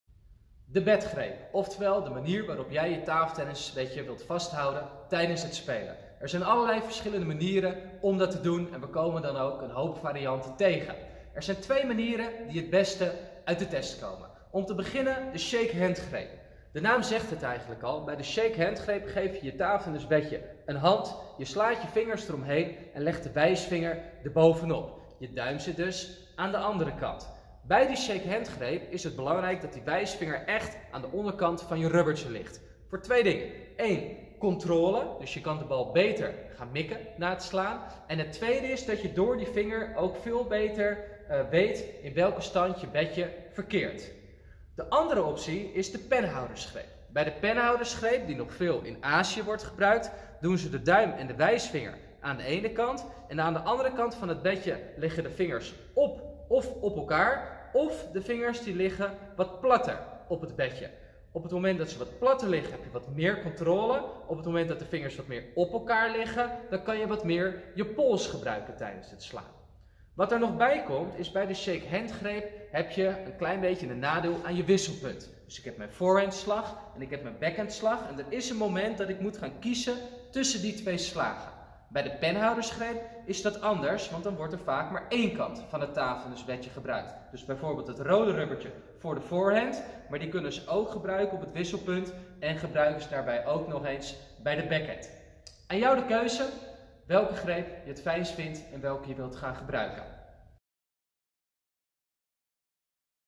The Grip - De Batgreep | Table Tennis Techniques
How to hold your table tennis bat. There are all kinds of ways to do it, we see a lot of different styles. There are two main ways to to hold the bat. It’s up to you which one you prefer and which one you want to use!
演奏时握住乒乓球拍的方式。有各种各样的方法可以做到，我们看到了许多不同的风格。握住桨的主要方法有两种。首先，握手。这个名字不言而喻，您与桨握手，将手指缠绕在手柄上，然后将手指放在桨上方。拇指在桨的另一侧。像这样握着桨时，重要的是将手指放在橡胶的底部。有两个主要原因。首先，控制时，击球时瞄准球更容易。第二个原因是因为有了手指，您对桨叶的角度感觉更。另一种选择是握笔的方式。使用这种在亚洲仍很流行的握法，您可以将拇指和食指放在一侧，另一侧将手指放在彼此的上方或上方，或者将手指放得更平一些。如果将它们放置得更平，则可以控制的更多，将它们更多地放在彼此之上时，击打时可以多用一些手腕。使用握手手柄时，在切换点上有一个小的缺点。我可以用正手击球，也可以用反手击球，但是在此之间需要片刻之间进行选择。虽然握笔时有区别，但大多数情况下它们仅使用桨叶的一侧。因此，例如红色橡胶用于正手，但您也可以在切换点和反手使用它。由您决定选择哪一个以及使用哪个！